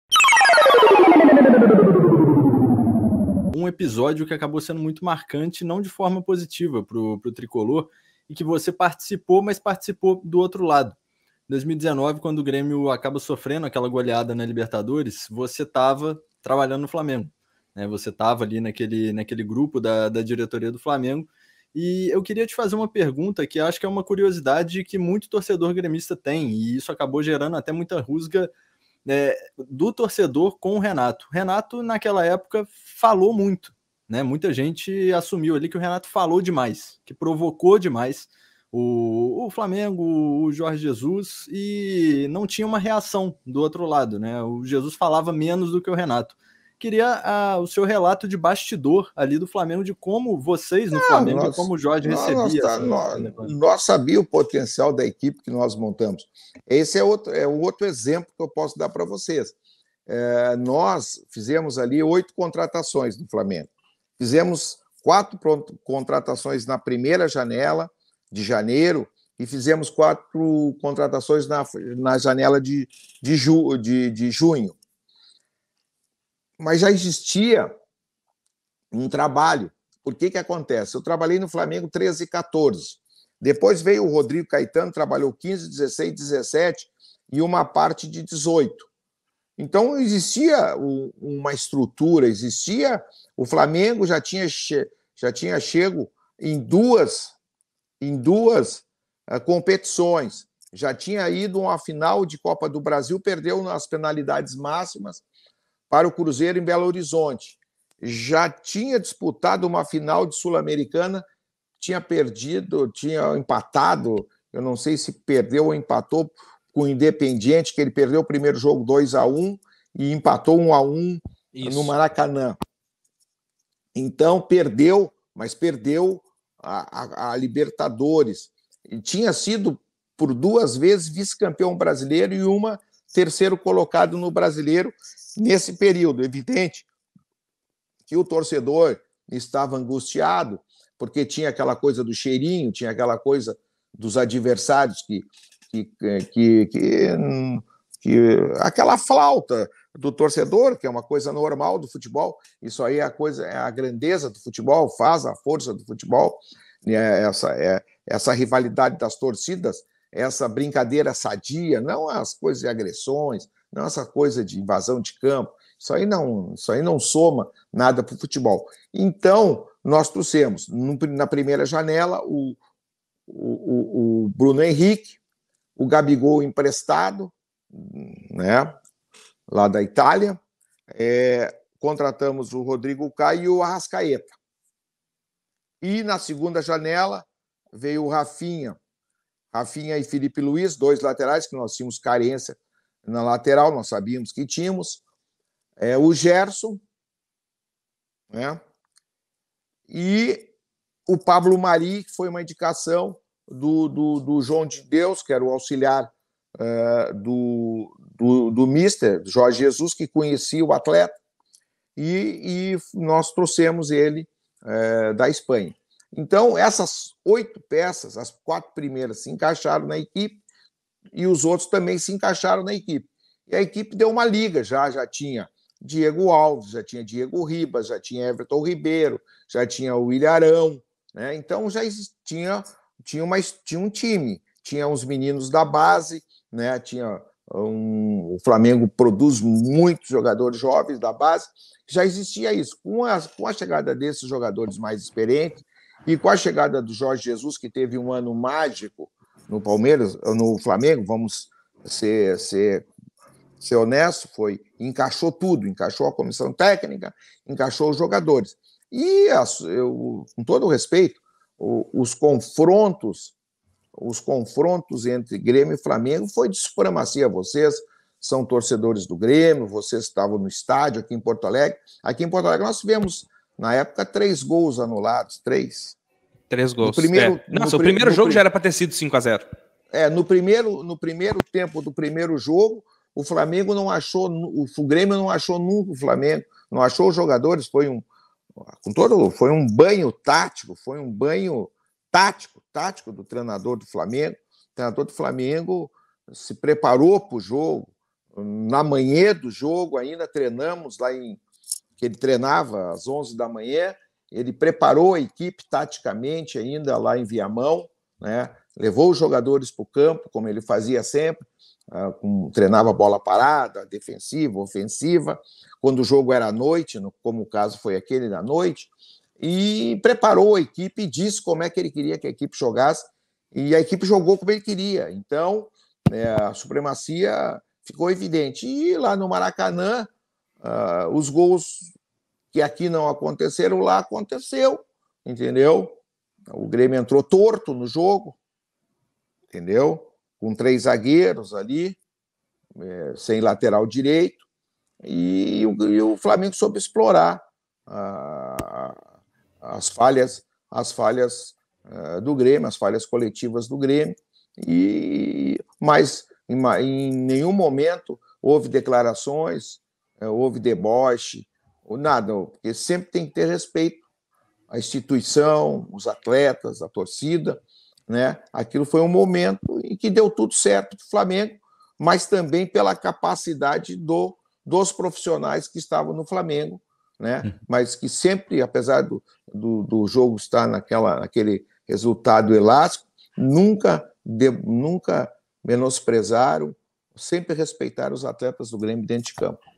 EXCLUSIVO: Pelaipe revela responsável pela goleada sofrida para o Fla em 2019
Um dos episódios negativos mais marcantes da história recente do Grêmio foi o duelo diante do Flamengo na Libertadores de 2019. Muitos atribuem a culpa a Renato Gaúcho. O fato é que o resultado marcou uma virada de chave na situação do Tricolor até os dias atuais, culminando no trágico ano de 2021.

Paulo Pelaipe, histórico dirigente do Grêmio, trabalhava no Flamengo à época, e revelou ao PG os bastidores da relação de Renato com Jorge Jesus.